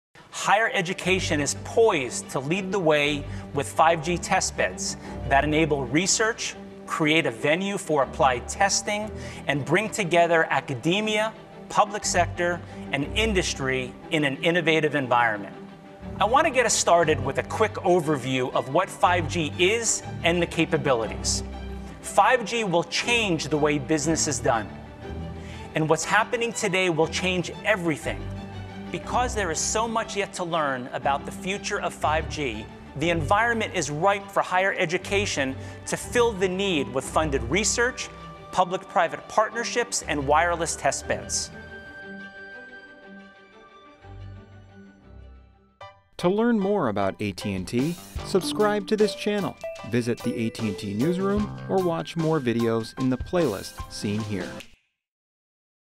Higher Education and 5G Testbeds Are Working Together
AT&T’s Matt Hickey shares how this partnership will help to learn about the future of 5G.